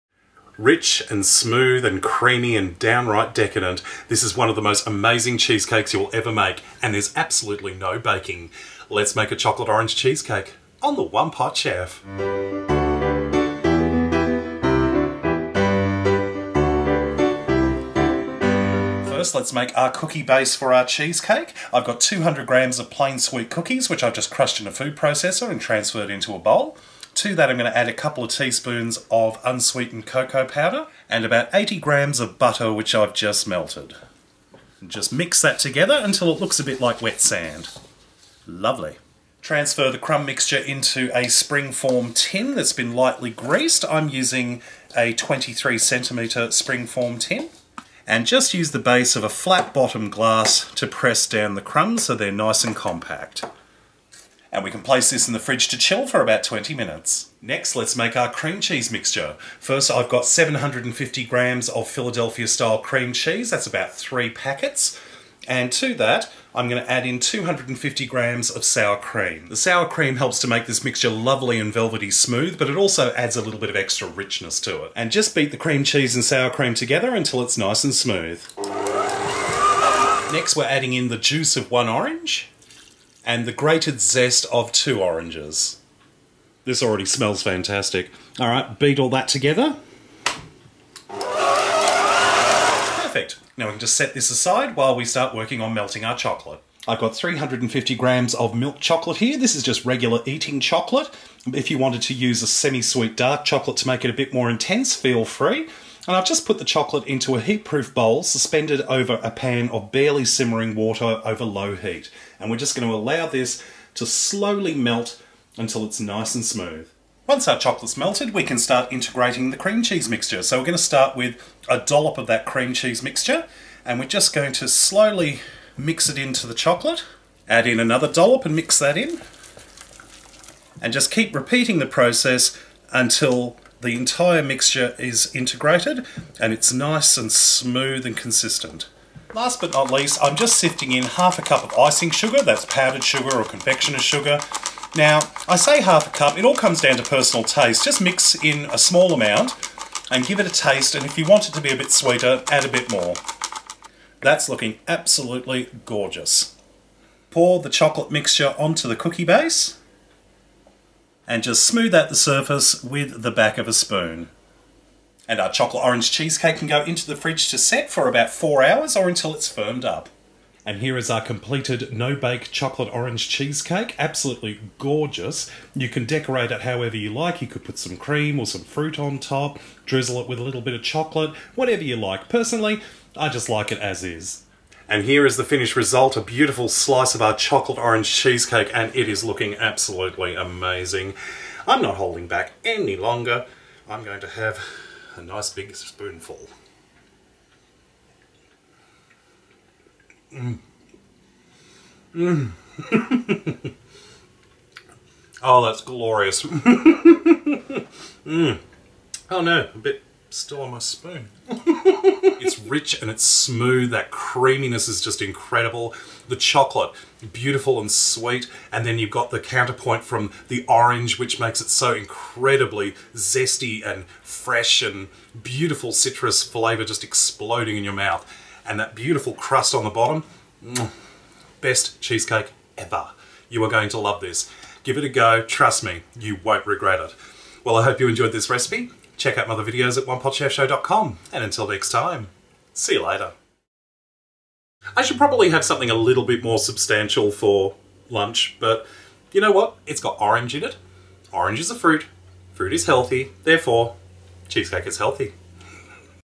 No Bake Chocolate Orange Cheesecake | One Pot Chef
No Bake Chocolate Orange Cheesecake is a decadent, rich dessert - perfect for parties and celebrations. Cream cheese, sour cream, sugar and orange zest are combined, then folded into melted milk chocolate, Smoothed over a crushed cookie base and chilled until firm, this amazing cheesecake is simple to make, but very impressive - give it a go!

INGREDIENTS IN THIS DISH:

200g of Plain Sweet Cookies (crushed)
80g of Butter (melted)
2 Teaspoons of Unsweetened Cocoa Powder
750g of Cream Cheese (about 3 blocks / packets)
250g of Sour Cream
1/2 to 1 Cup of Icing Sugar (powdered sugar / confectioners sugar)
Juice of 1 Orange
Zest of 2 Oranges
350g of Milk Chocolate

Chilling Time: About 3 - 4 hours

ALL MEASUREMENTS GIVEN ARE AUSTRALIAN STANDARD METRIC
(Look up Google for a conversion chart if using Imperial)